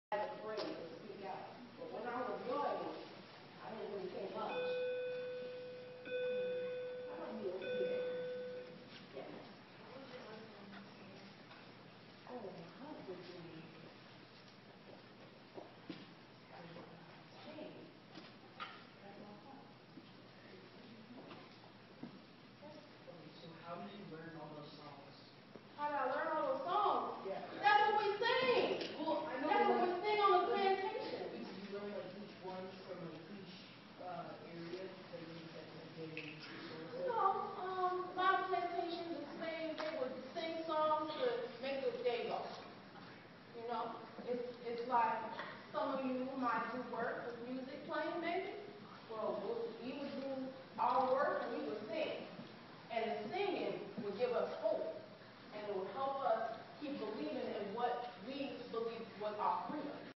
Danielle Charlestin of the League of Woman Voters of Sgta. Co. portrays Sojuner Truth in a performan
Danielle Charlestin of the League of Woman Voters of Sgta. Co. portrays Sojuner Truth in a performance at Shen HS Wst

By: Glenn Griffith
Published on: March 2, 2015